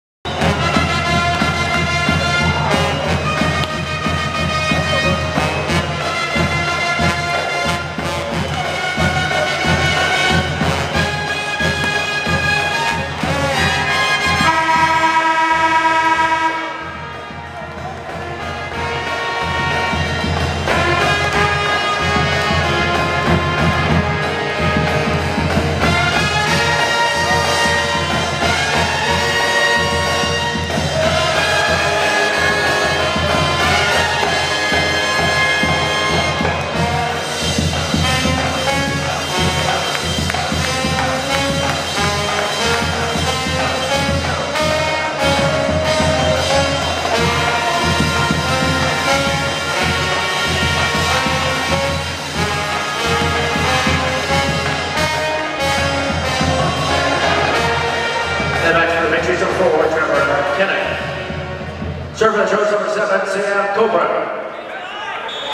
trojancandy.com: Watch our USC Song Girls Perform at the Men's Volleyball Match vs George Mason
At trojancandy.com:  Watch our USC Song Girls perform at the Men's Volleyball match vs George Mason.  Read the entire story and see more pictures and videos at trojancandy.com.